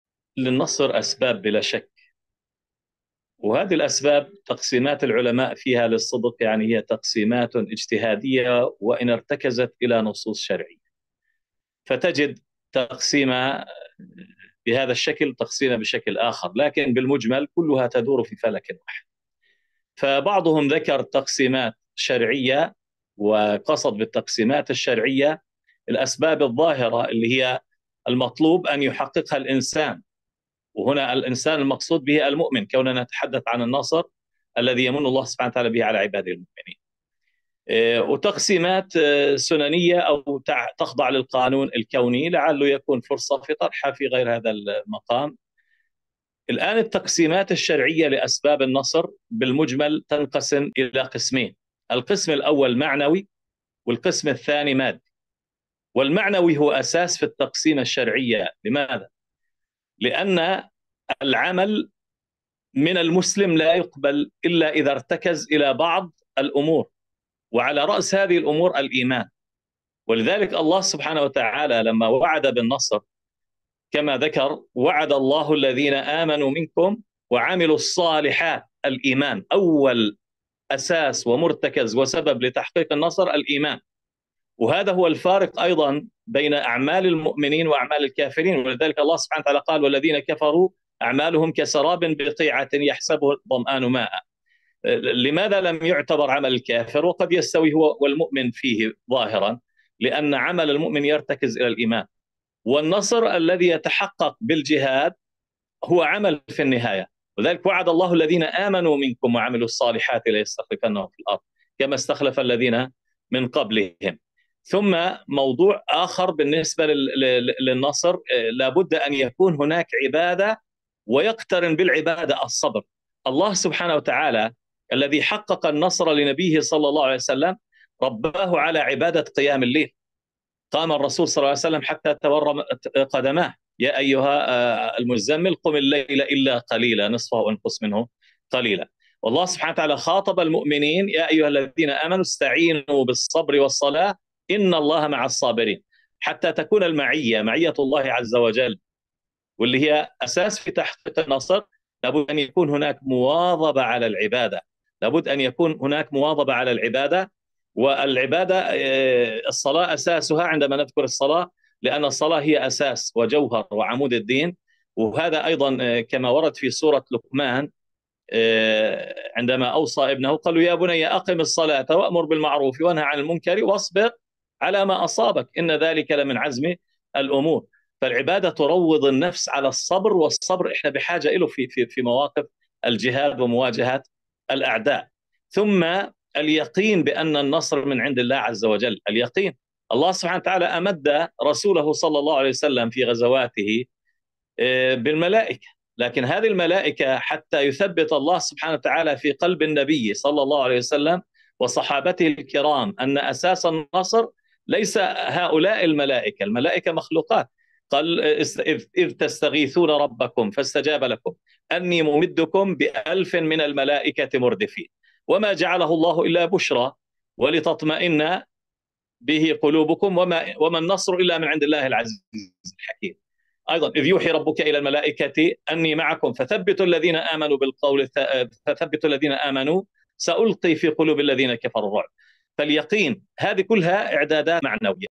"ما هي الأسباب المعنوية للنصر كما جاءت في الكتاب والسنة والسيرة النبوية؟" د. إياد جبور
للمزيد من المقاطع المرئية والمشاهد والمقابلات مع الدعاة والعلماء ومشايخ فلسطين
يرجى زيارة موقعنا على الرابط التالي